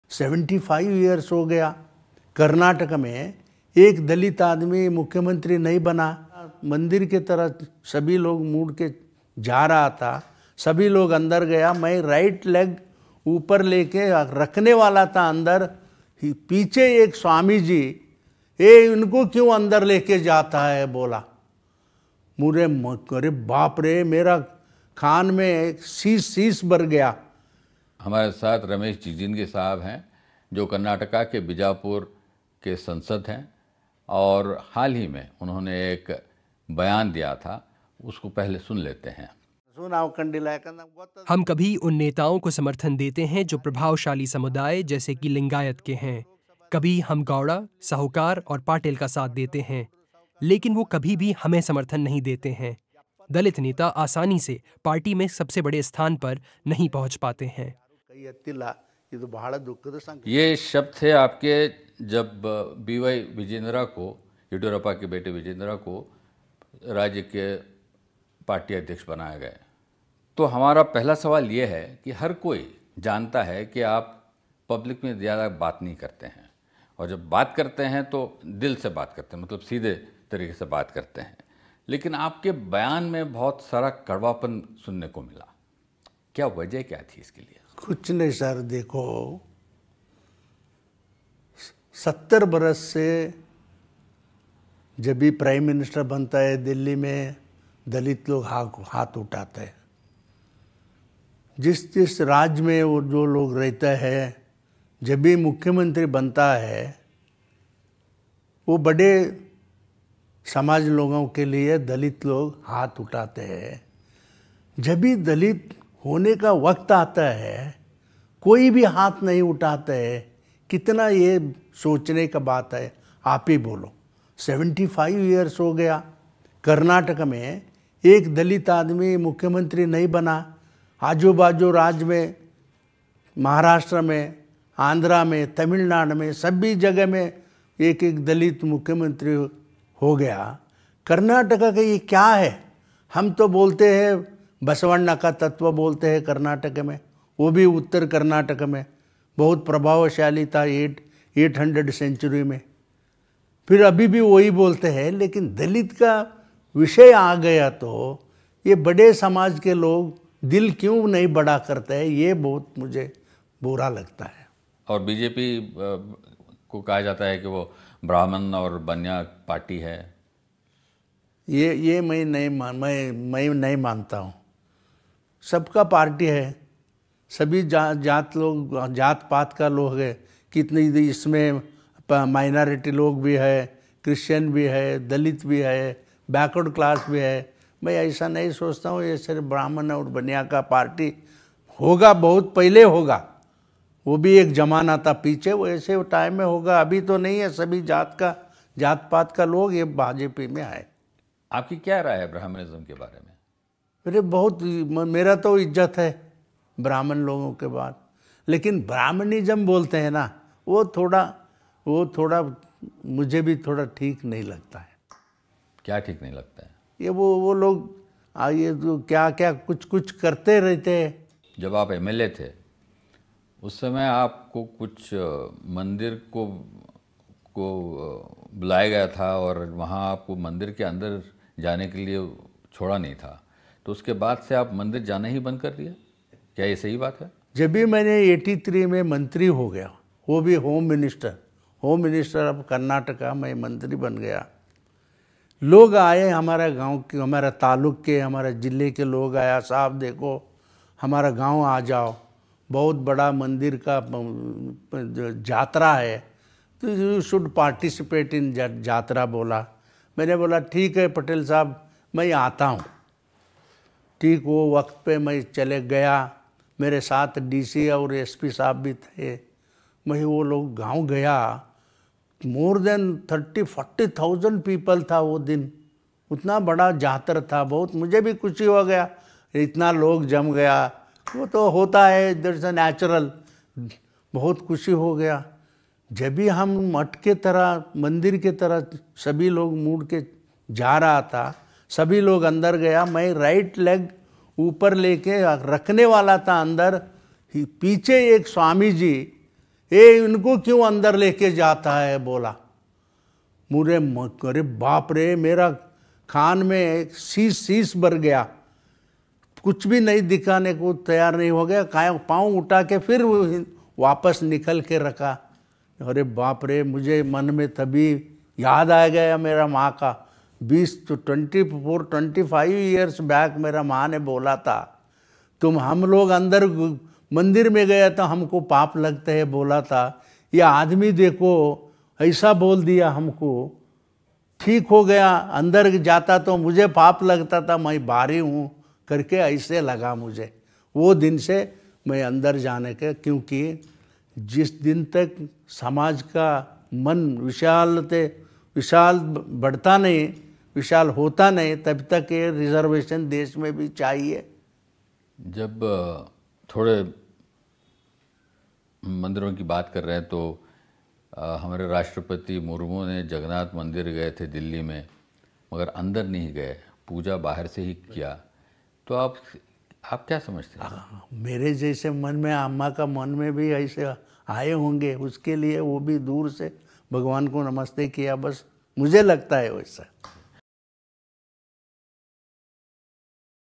Ramesh Jigajinagi Interview: BJP MP ਮੁਤਾਬਕ ਦਲਿਤ ਨੇਤਾ ਕਦੇ ਵੀ ਉੱਚ ਅਹੁਦਿਆਂ 'ਤੇ ਨਹੀਂ ਪਹੁੰਚ ਸਕਦੇ | 𝐁𝐁𝐂
ਰਮੇਸ਼ ਜਿਗਾਜਿਨਾਗੀ ਕਰਨਾਟਕ ਨੇ ਬੀਜਾਪੁਰ ਤੋਂ ਭਾਜਪਾ ਦੇ ਸੰਸਦ ਮੈਂਬਰ ਹਨ। ਹਾਲ ਹੀ 'ਚ ਉਨ੍ਹਾਂ ਨੇ ਬਿਆਨ ਦਿੱਤਾ ਸੀ ਕਿ ਦਲਿਤ ਨੇਤਾ ਕਦੇ ਵੀ ਪਾਰਟੀ 'ਚ ਉੱਚ ਅਹੁਦਿਆਂ 'ਤੇ ਨਹੀਂ ਪਹੁੰਚ ਸਕਦੇ। ਉਨ੍ਹਾਂ ਦੇ ਇਸ ਬਿਆਨ ਤੋਂ ਬਾਅਦ ਭਾਜਪਾ ਅੰਦਰ ਵੀ ਕਈ ਸਵਾਲ ਉੱਠਣੇ ਸ਼ੁਰੂ ਹੋ ਗਏ ਹਨ। ਆਖਿਰ ਰਮੇਸ਼ ਜਿਗਾਜਿਨਾਗੀ ਨੇ ਅਜਿਹਾ ਕਿਉਂ ਕਿਹਾ, ਦੇਸ਼ 'ਚ ਜਾਤੀ ਜਨਗਣਨਾ 'ਤੇ ਉਨ੍ਹਾਂ ਦੇ ਕੀ ਵਿਚਾਰ ਹਨ ਅਤੇ ਅਜਿਹਾ ਕੀ ਹੋਇਆ ਜਿਸ ਤੋਂ ਬਾਅਦ ਉਨ੍ਹਾਂ ਨੇ ਮੰਦਰ ਨਾ ਜਾਣ ਦਾ ਫੈਸਲਾ ਕੀਤਾ। ਬੀਬੀਸੀ ਲਈ ਰਮੇਸ਼ ਜਿਗਾਜਿਨਾਗੀ ਨਾਲ ਇਮਰਾਨ ਕੁਰੈਸ਼ੀ ਦੀ ਇਹ ਖਾਸ ਗੱਲਬਾਤ ਦੇਖੋ।
ਕੈਮਰਾ: ਅਲਫੋਂਸ ਵਿਮਲਰਾਜ
 ਐਡਿਟ: ਸਿਰਾਜ ਅਲੀ